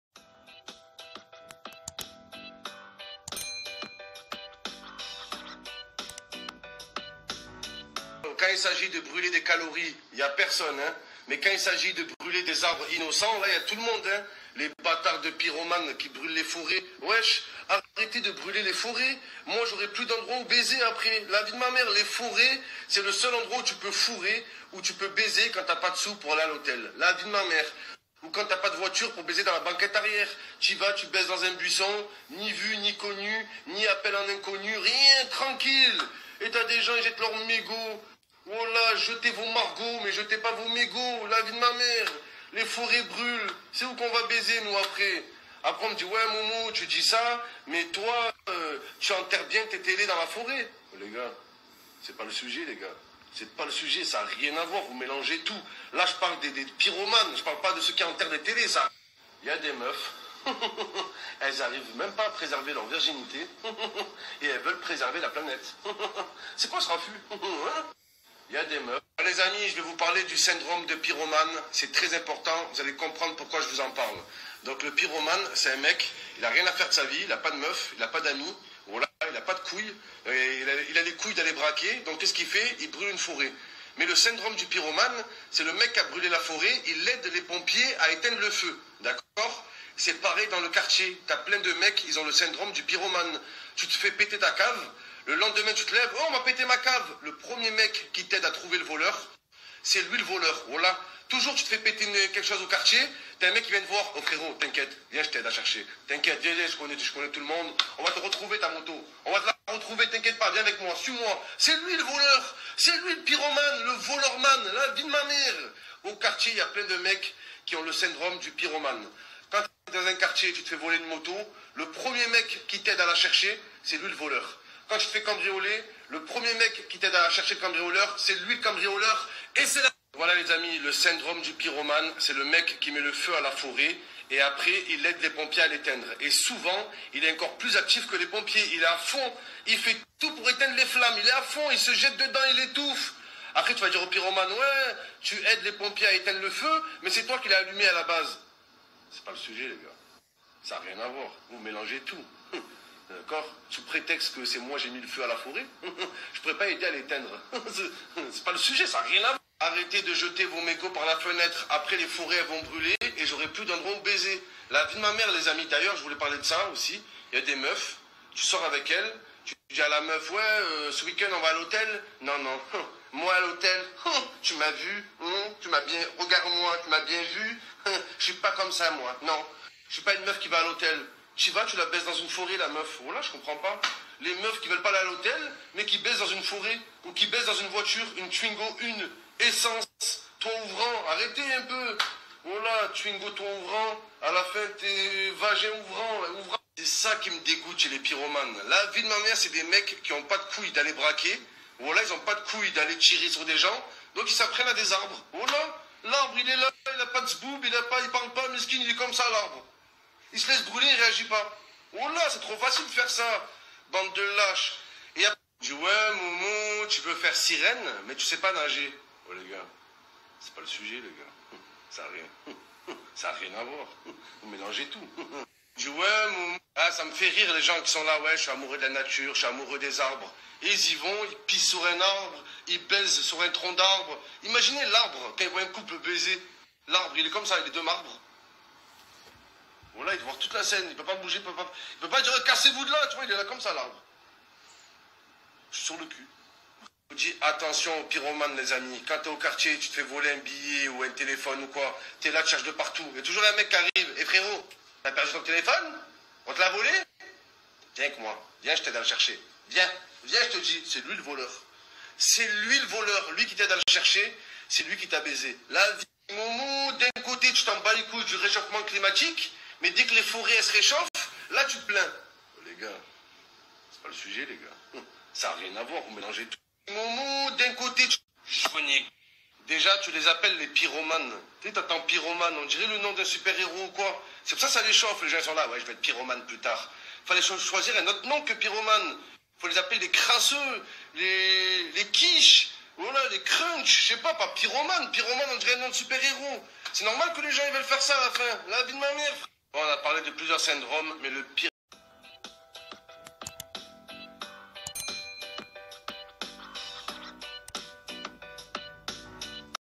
MOHAMED HENNI POUSSE UN COUP DE GUEULE A PROPOS DES PYROMANES 😱
Comme vous pouvez le voir dans la vidéo le célèbre Youtubeur Mohamed Henni pouse un coup de gueule a propos des pyromanes 😱 nesite pas a vous abonnée a ma chaîne YouTube et activé la cloche de notification pour ne pas ratée aucune de mes vidéos je vous souhaite tous un excellent visionnage merci !